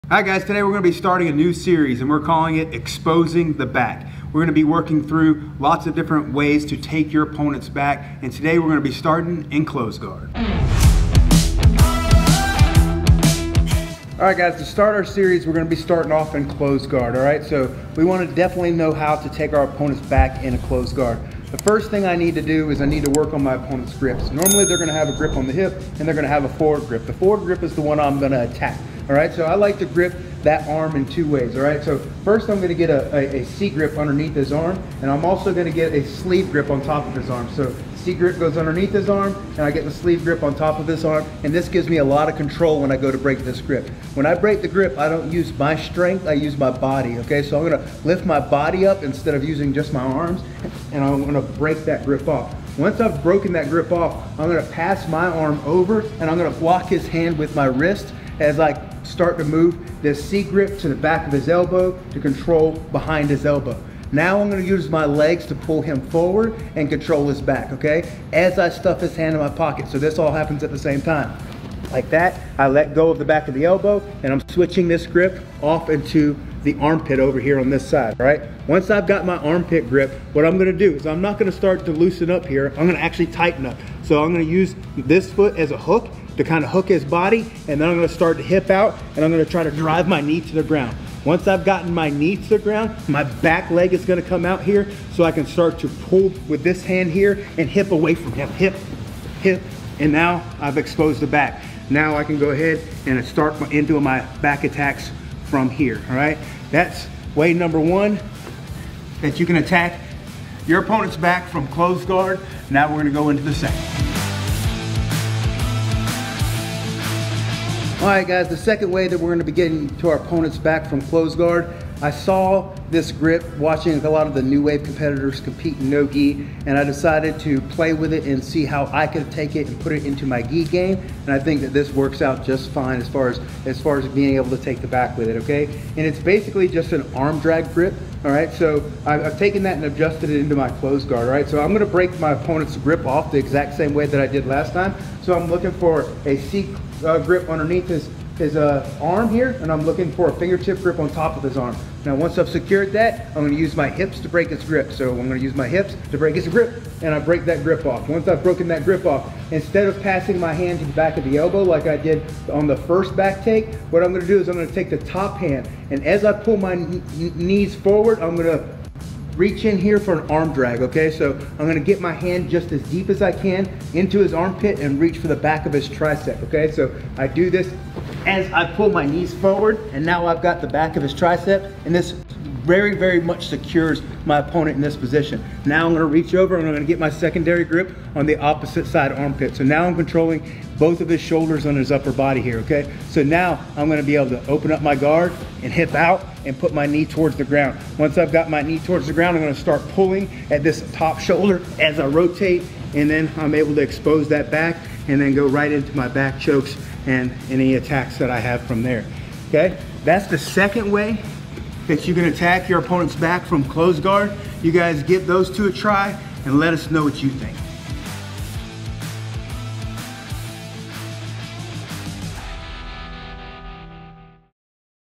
Exposing the Back - New Wave Grip - Closed Guard
Exposing the back
Getting to the back of your opponent is the most important skill in Jiu-Jitsu. It limits them to mainly defending, while you have an array of attacks and transitions available to you. In this series we will show many paths to the back from different positions as well as some submissions on the way to getting there.